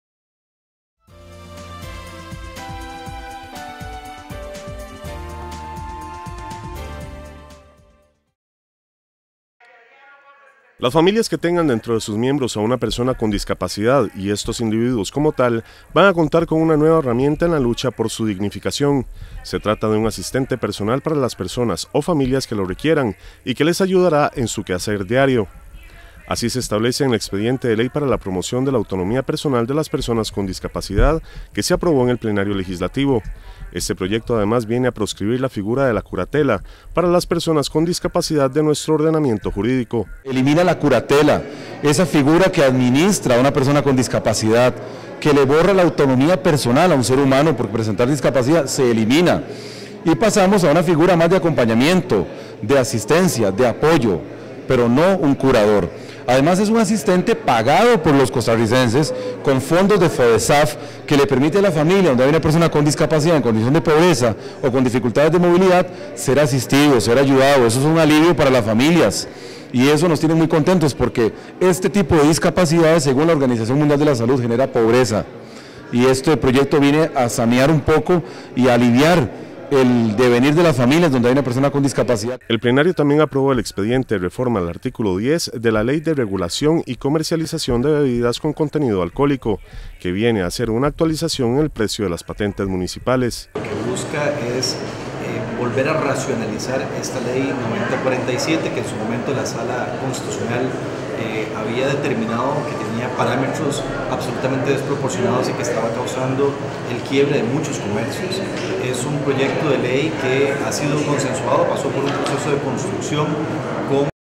APRUEBAN LEY DE AUTONOMÍA PARA PERSONAS CON DISCAPACIDAD
Las familias que tengan dentro de sus miembros a una persona con discapacidad y estos individuos como tal, van a contar con una nueva herramienta en la lucha por su dignificación.